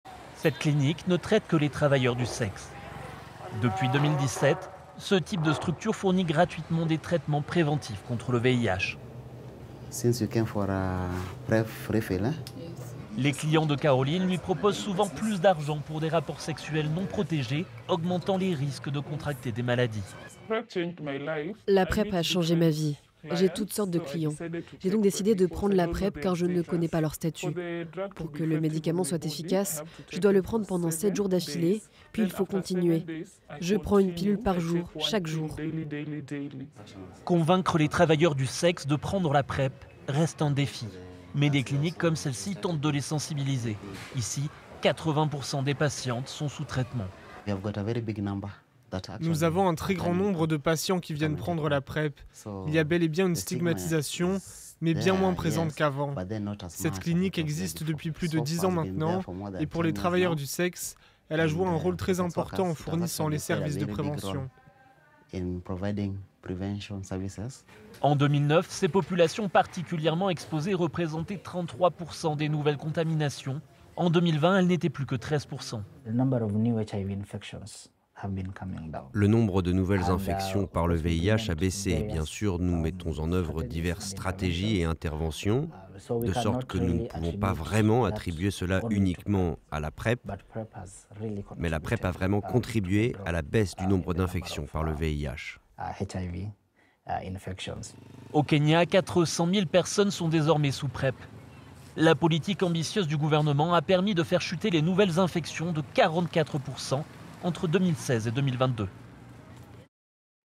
VIH : au Kenya, les travailleurs du sexe moins exposés grâce aux traitements préventifs gratuits
Chaque année, le 1er décembre marque la Journée mondiale de lutte contre le sida. Au Kenya, des traitements préventifs contre le VIH sont accessibles gratuitement depuis 2017. Une politique qui contribue à la baisse des nouvelles infections, notamment chez les travailleurs et travailleuses du sexe, particulièrement à risque de contracter le virus. Reportage.